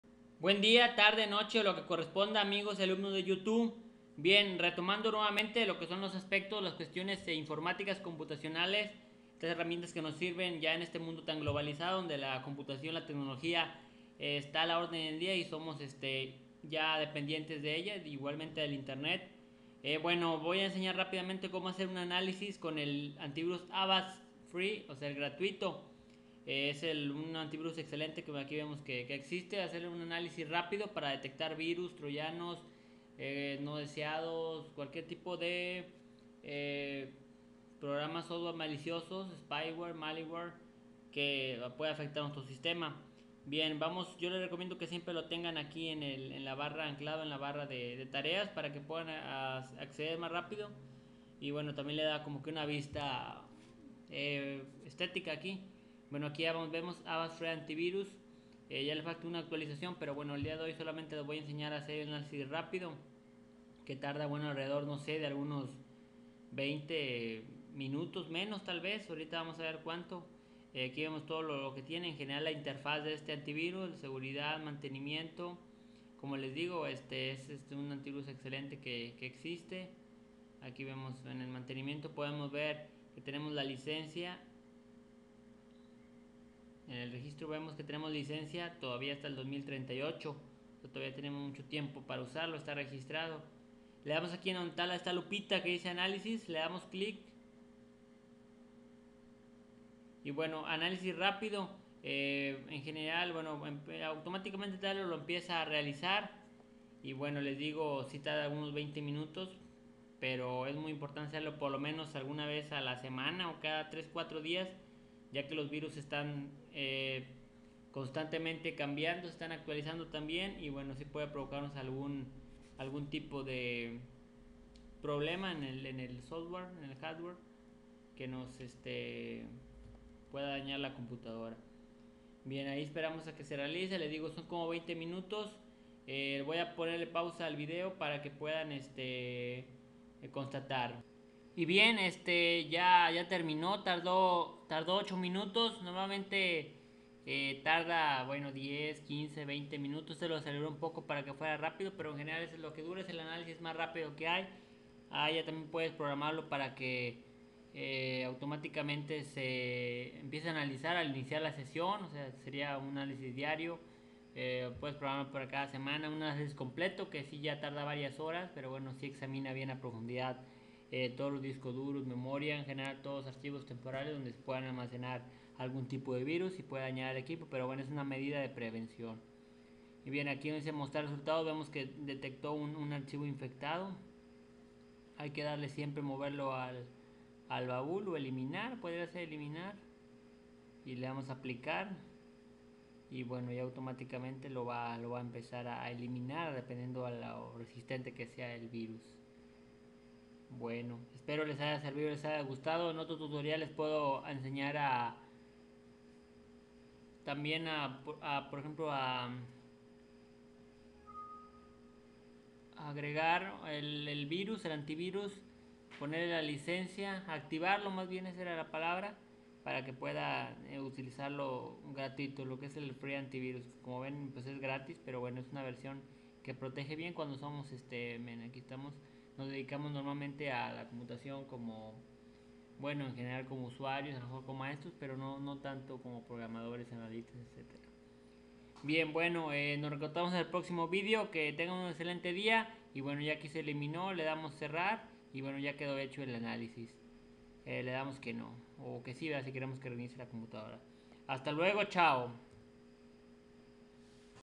Como hacer un análisis en AVAST FREE
Con su Maestro de Informática y Gramática aprenderemos rápidamente a efectuar un Análisis en el Antivirus AVAST, para detectar y eleminar virus, troyano o algún archivo o programa no deseado que pueda dañar el equipo.